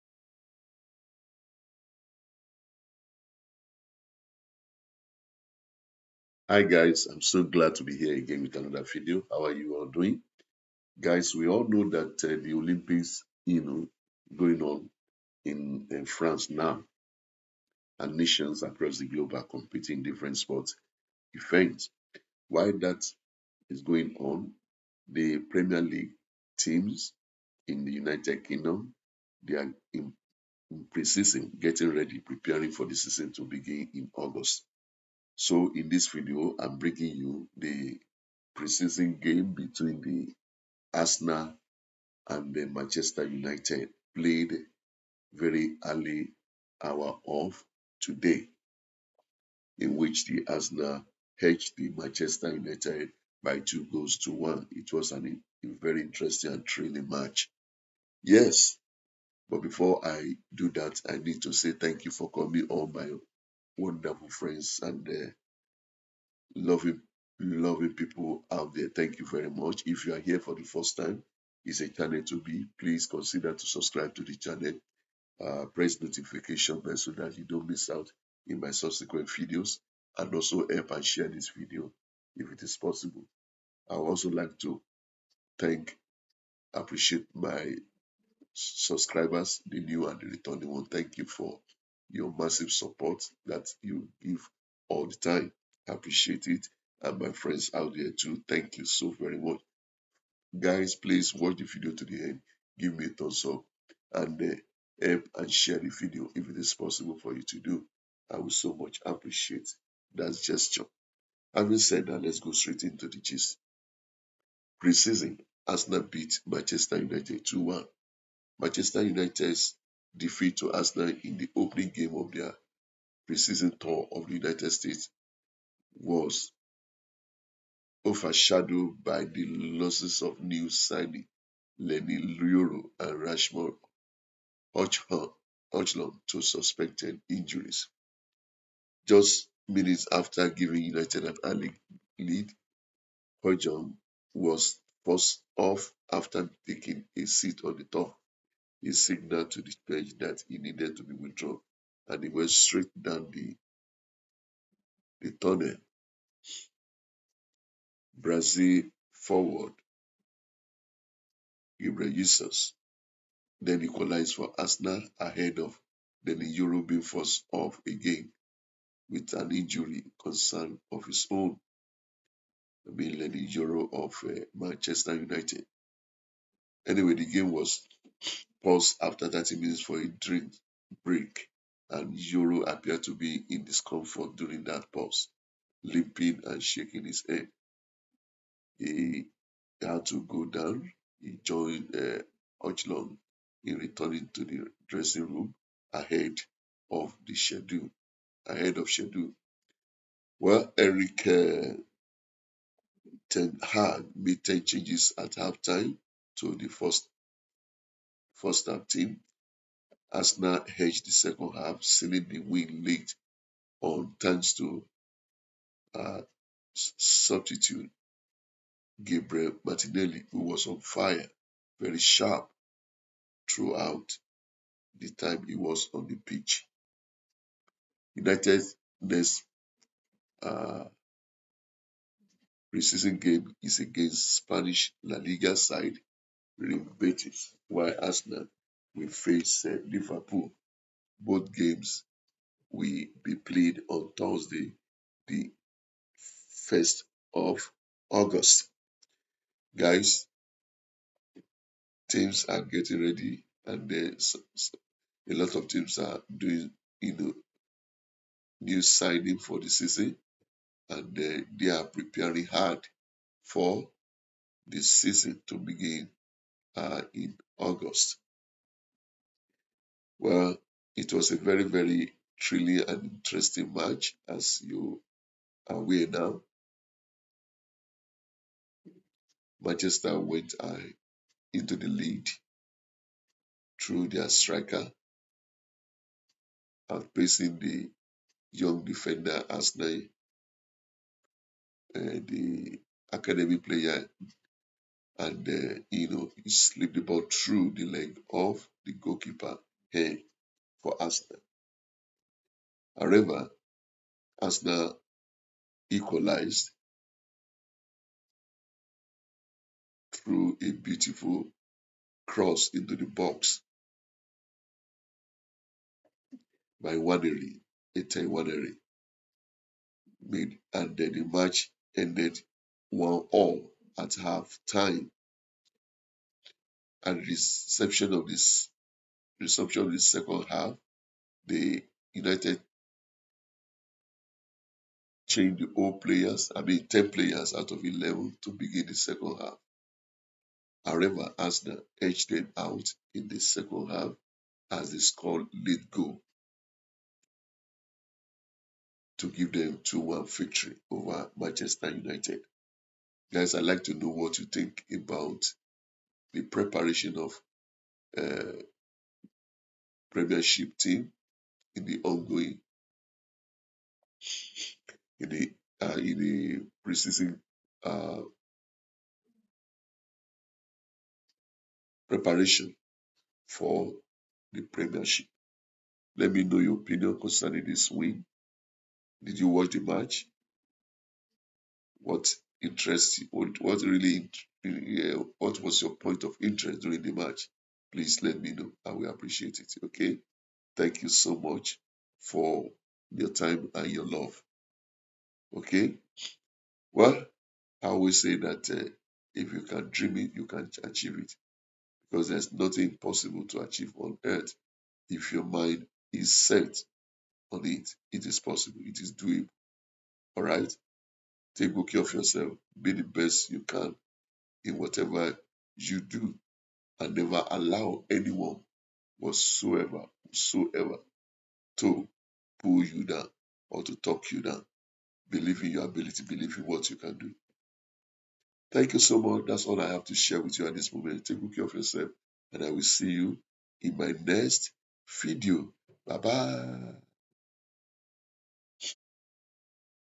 Arsenal Triumphs Over Manchester United 2-1 In Pre-season Showdown!arsenalfc
Welcome guys to my channel. In this video, Arsenal beat man United in a pre-season game 2-1. Thanks.